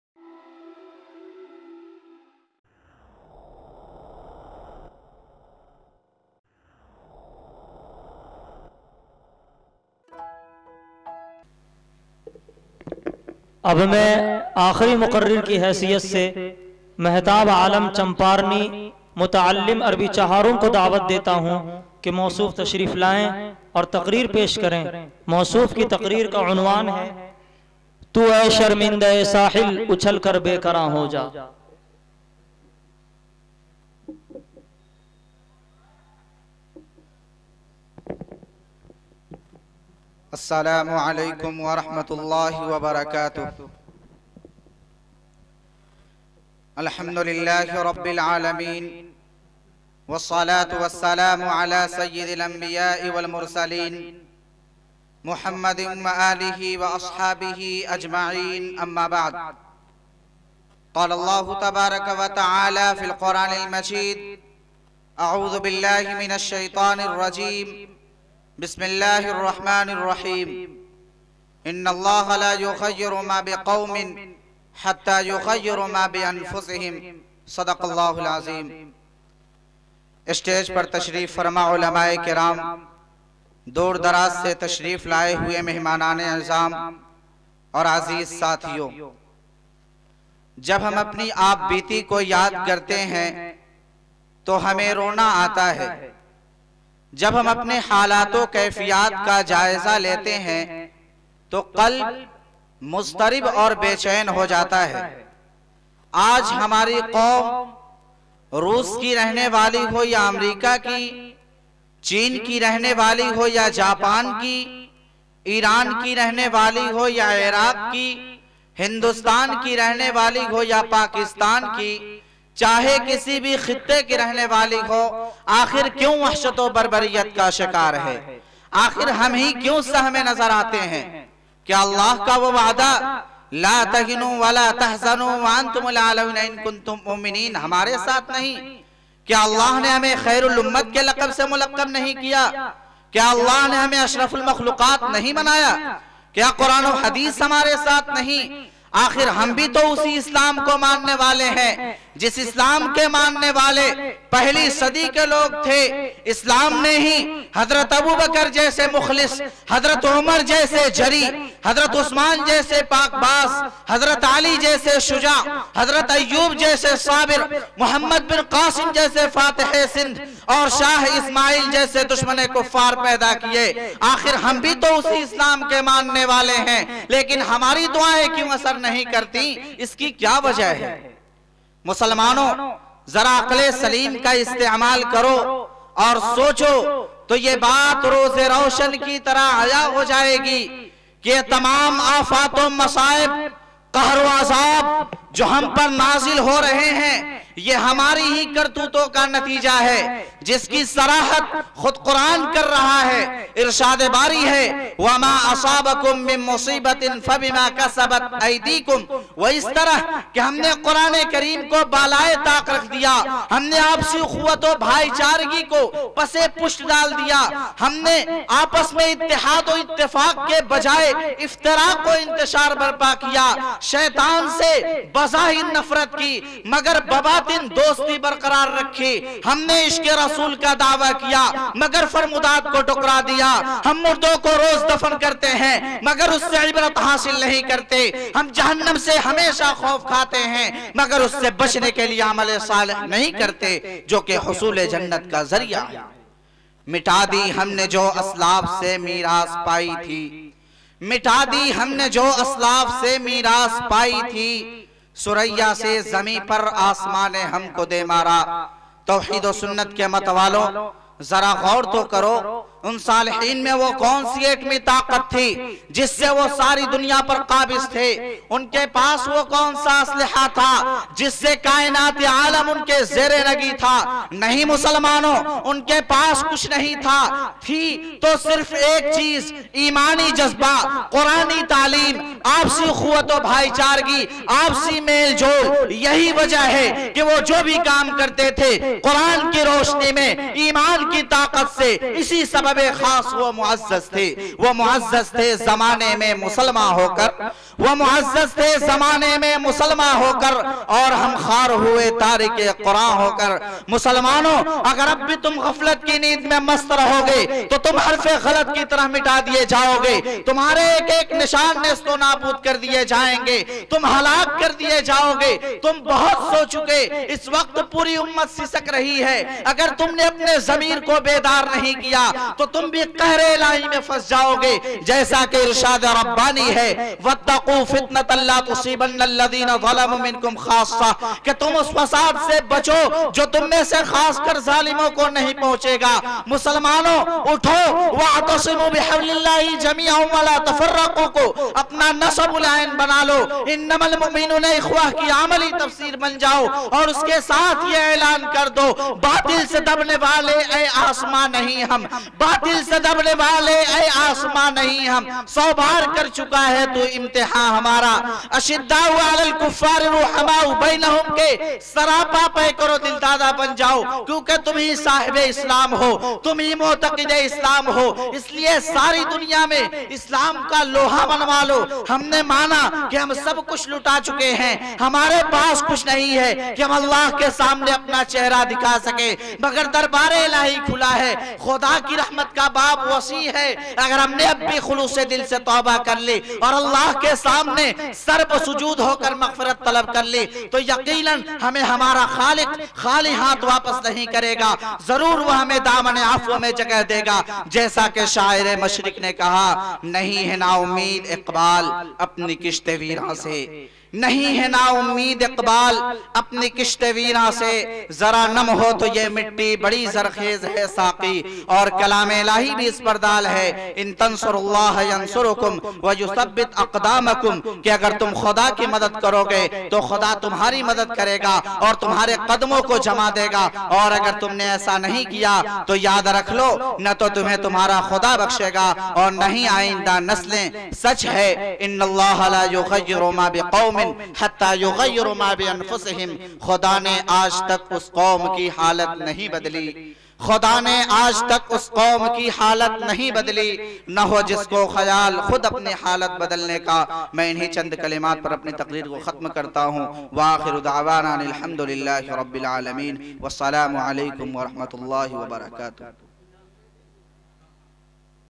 Mahtab Alam Champarni Jalsa Madarsa Mumbaul Uloom Khairabad Mau 08-02-2024 JK Mushaira Media
Mahtab Alam Champarni Jalsa Madarsa Mumbaul Uloom Khairabad Mau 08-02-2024
Nazim e Mushaira: Mohammad Rafey Hasibi
Zere Sadarat: Maulana Abdur Rab Sab
Zere Sarparasti: Maulana Abdul Hayee Sb
Isteqbaliya: Alhaj Qari Abdul Jalil Sb
Venue: Madarsa Mumbaul Uloom Khairabad Mau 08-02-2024